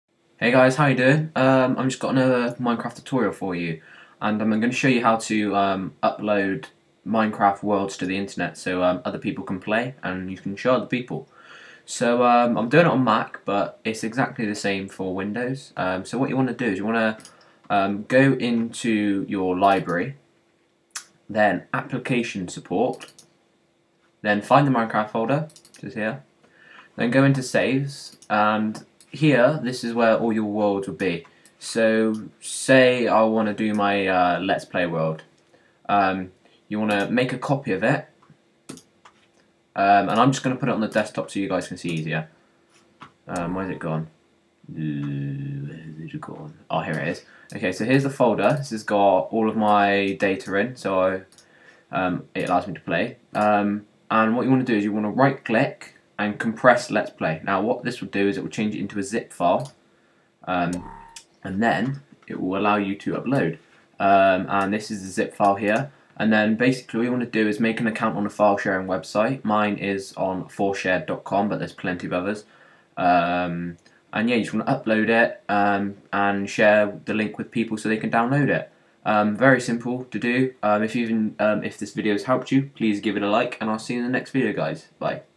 Minecraft:How To Upload Minecraft Worlds To The Internet
This video shows you how to upload your minecraft worlds to file sharing websites or for other uses, it is on mac but is the same on windows, very easy to do.